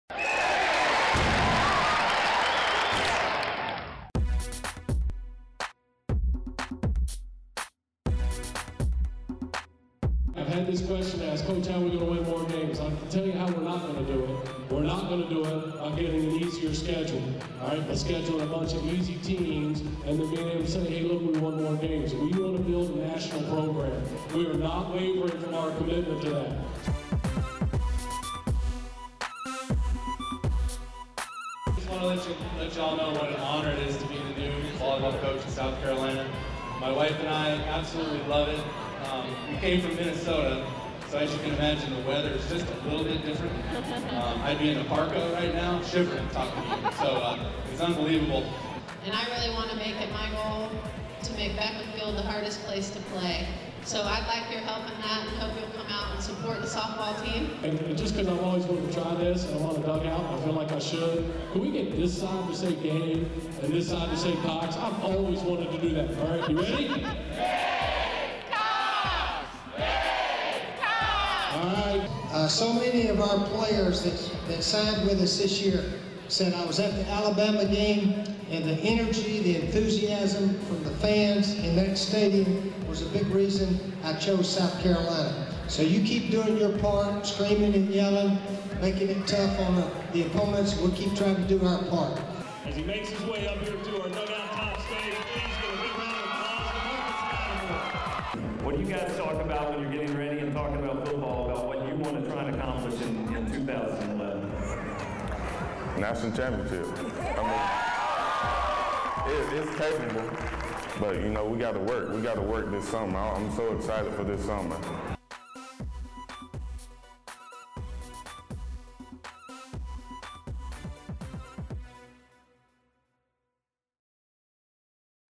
Gamecock Fan Fest Tour - Greenville
The Gamecock Fan Fest Tour presented by the Gamecock Club continued to Greenville at Fluor Field, home of the Greenville Drive. Fans heard from several coaches, took pictures with championship trophies, Cocky and the cheerleaders, and more. The Gamecock Fan Fest Tour:
May 11 - Charlotte Knights Stadium
May 16 - Charleston RiverDogs Stadium
May 17 - USC Aiken Convocation Center
May 18 - Greenville Drive Stadium
May 24 - USC Sumter Nettles Building
May 25 - Darlington Raceway